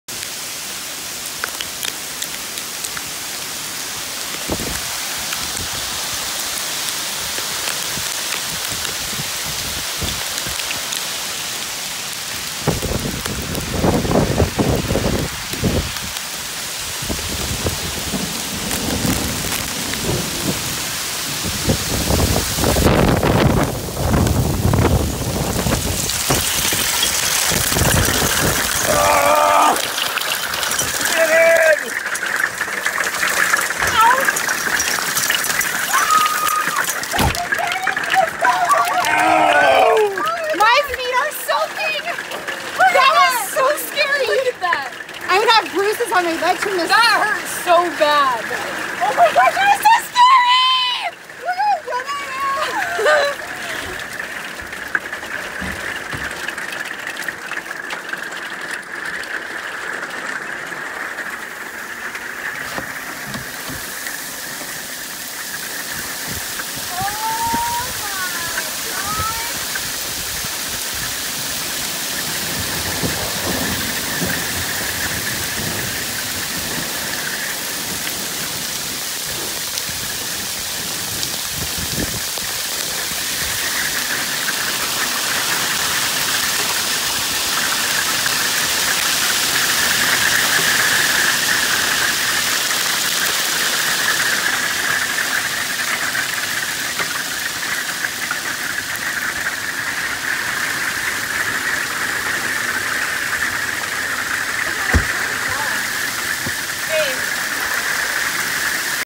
Hail @ Bristlecone Pine Forest
Crazy storm glad we had our truck camper